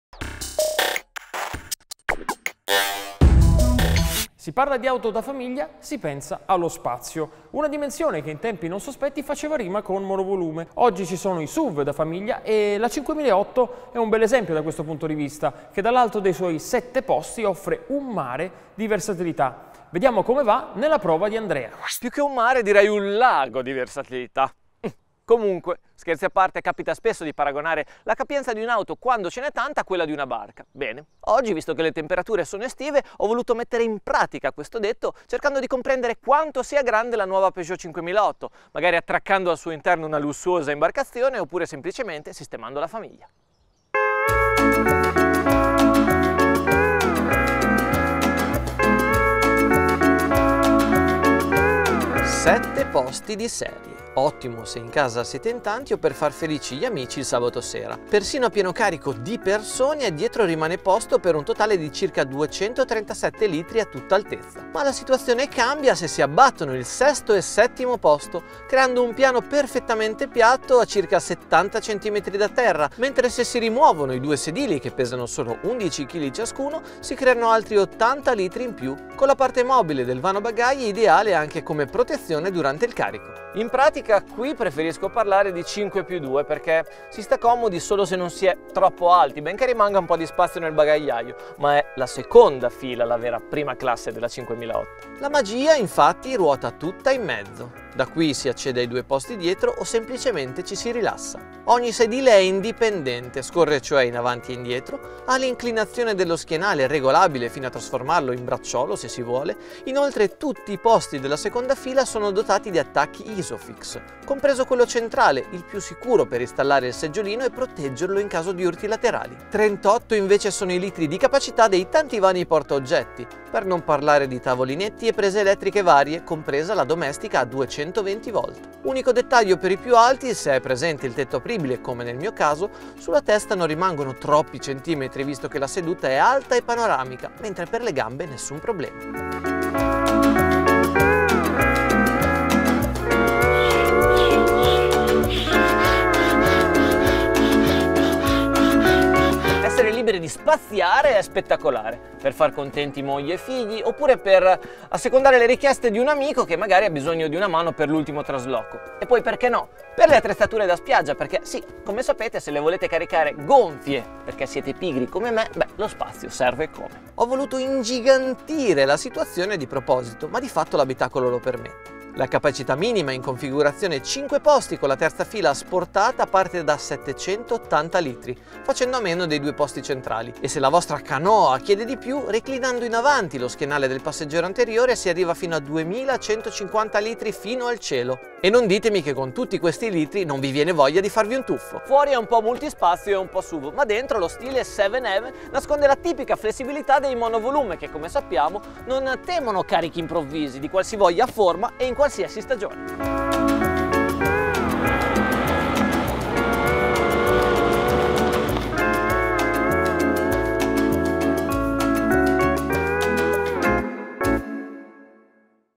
Un'auto... a prova di "barca"! | La Peugeot 5008 secondo Andrea
Con un'auto grande come il nuovo Peugeot 5008 è bello provare a giocare con lo spazio e caricare addirittura una barca. Andrea sfida la capacità del SUV francese fino ai suoi estremi facendo salire a bordo un natante molto particolare che trasforma la "5+2" in un perfetto compagno di avventure acquatiche. Via i sedili e dentro il gonfiabile per capire fino a che punto il nuovo 5008 può ampliare le proprie possibilità di trasporto.

Produzione: Edimotive